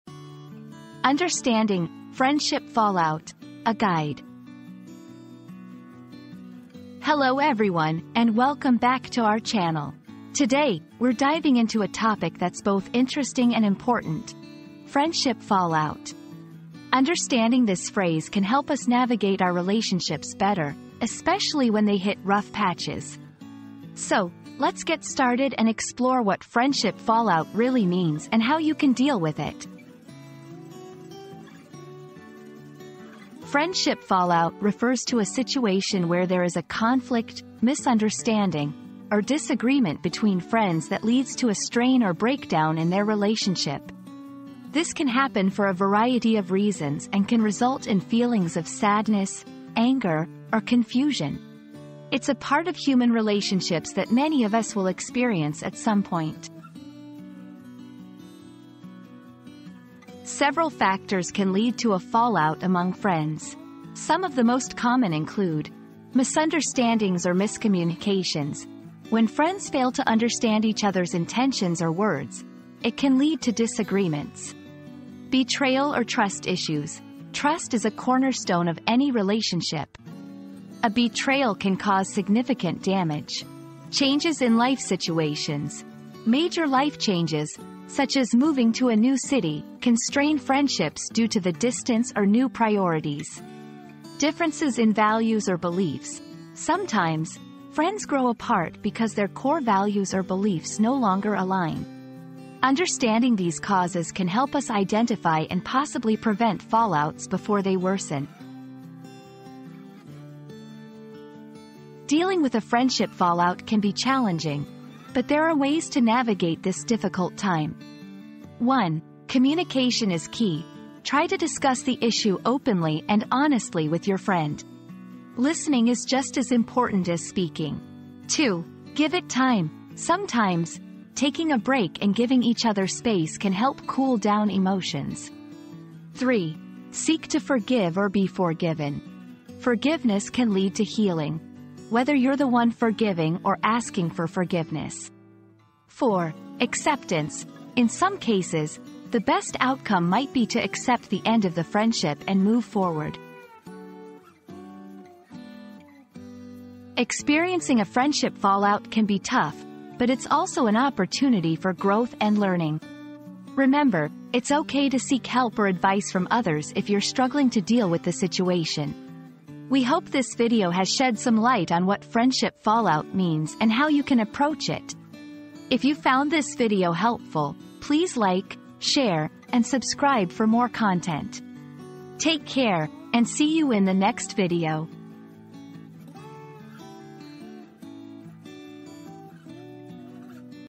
Understanding "Friendship Fallout": A Guide
00:00 • Introduction - Understanding "Friendship Fallout": A Guide
00:32 • What Does "Friendship Fallout" Mean?
00:59 • Common Causes of Friendship Fallout
01:51 • How to Deal With Friendship Fallout